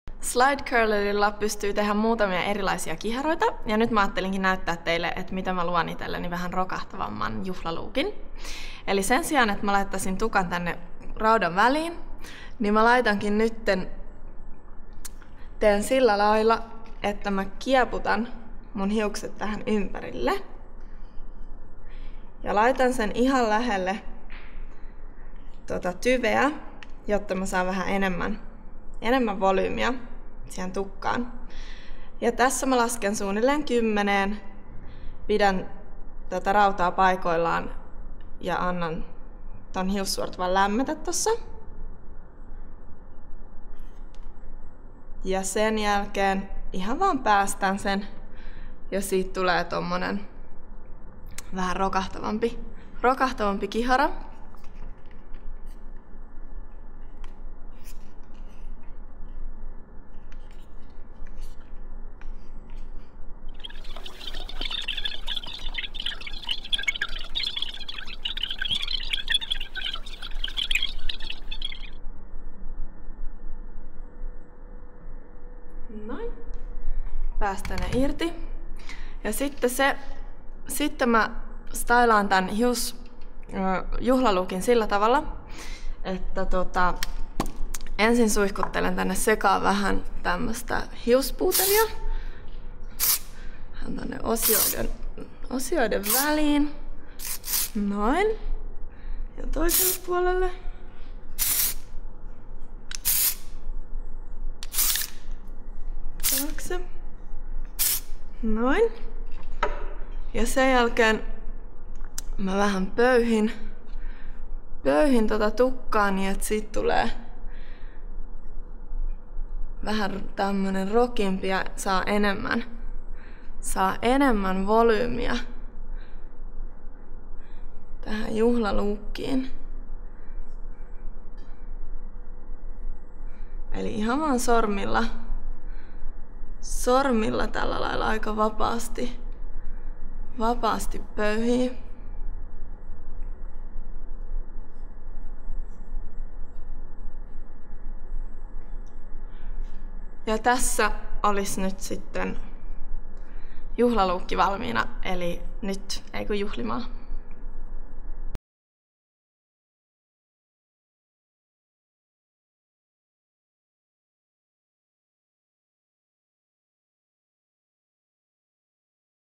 Hiusten kihartaminen Slide Curlerin avulla - party
Slide Curler on täysin uudenlainen OBH Nordican Björn Axén Tools -sarjaan kuuluva kiharrin. Täydelliset kiharat syntyvät, kun viet hiusosion joustavien levyjen väliin, kierrät kiharrinta kerran itsensä ympäri ja vedät tasaisesti kohti latvoja. Katso Linda Juholan ohjeet Slide Curlerille!